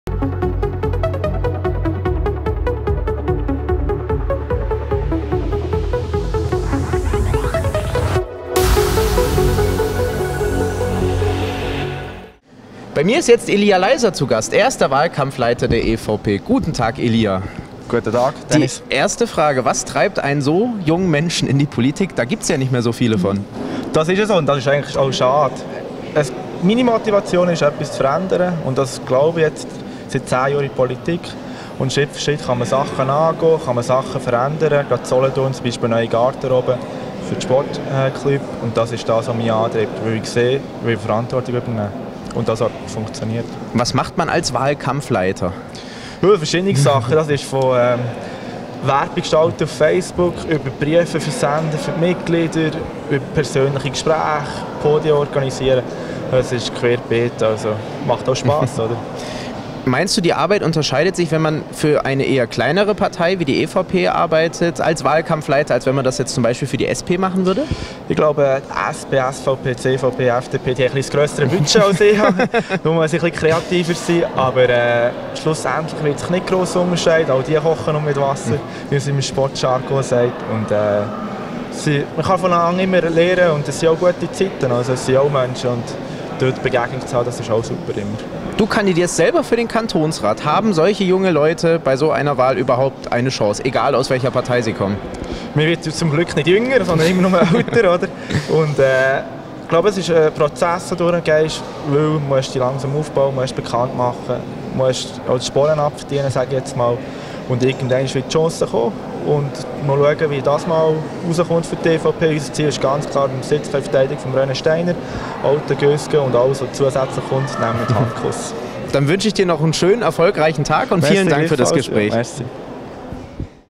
Wahlen Solothurn 2017 - Talk mit Elia Leiser
Auch die Jungpolitiker wie Elia Leiser von der EVP sind in der Rythalle vertreten.